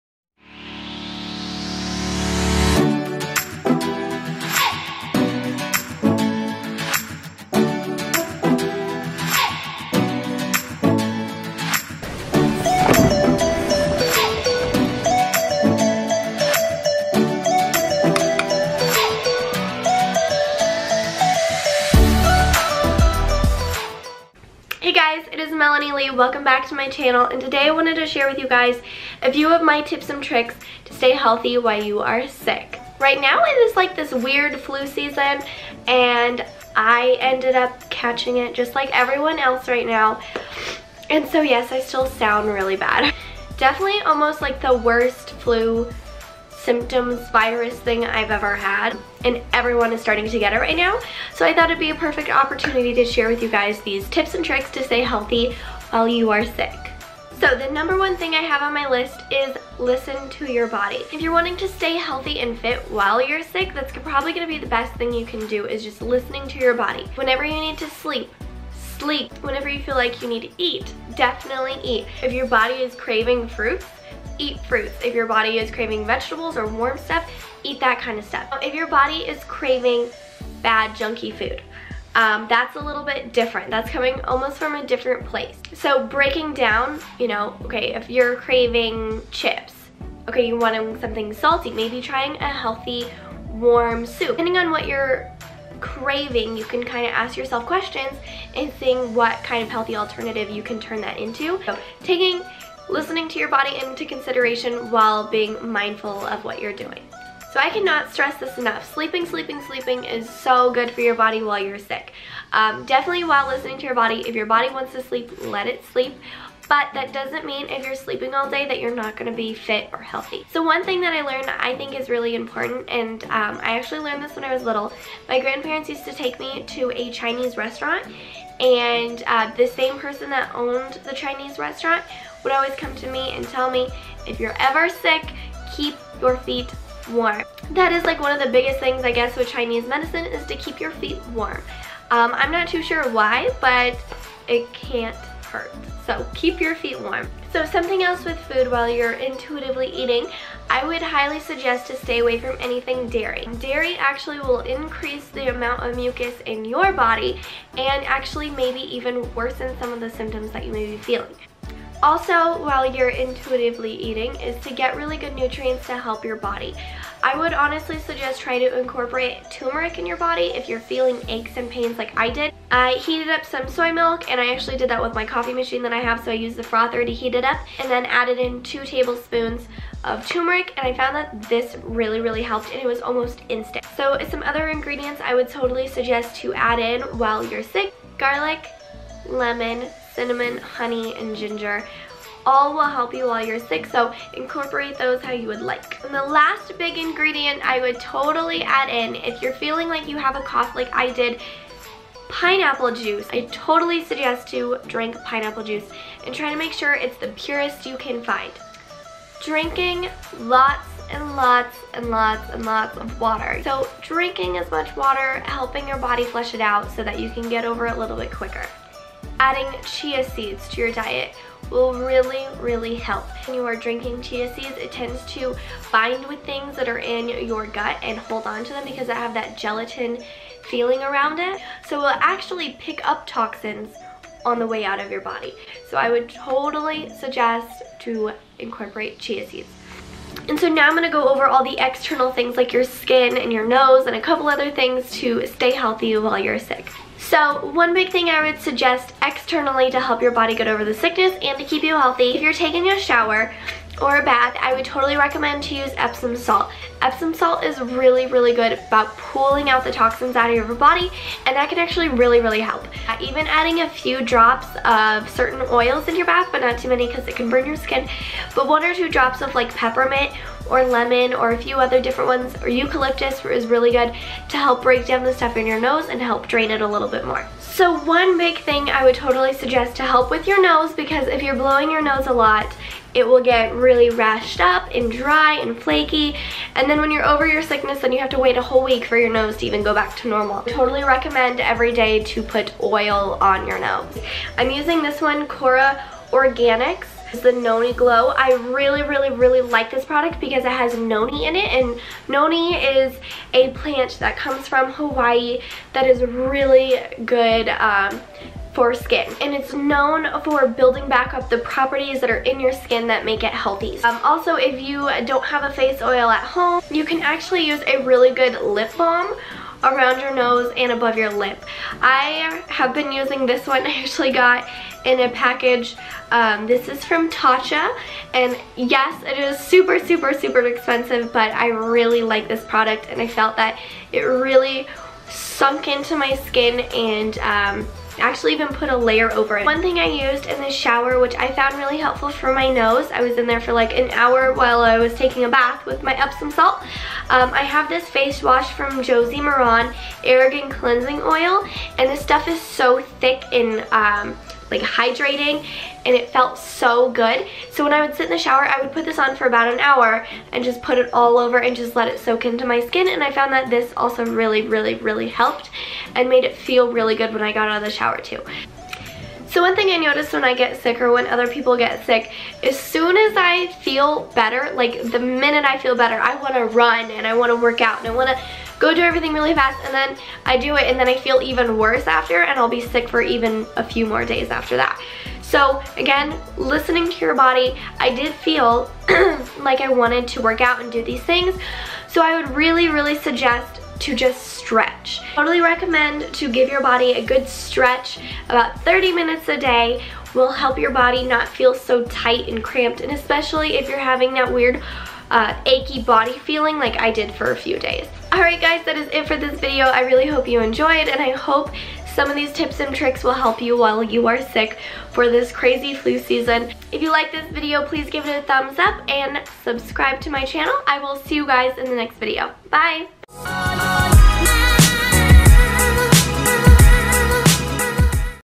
How To Stay HEALTHY While You're SICK!
Hey guys, so I really wanted to make this video because it seems like everyone is catching some type of cold or flu. I know it's super hard to stay healthy when you are sick, because usually you feel horrible and tired and not focused on nutrition for your body. I try to give some tips that hopefully help you stay healthy, which should consequently help your body fight off the sickness. I try to give advice on some foods and things you can do to help your immune system, and things I think you should stay away from!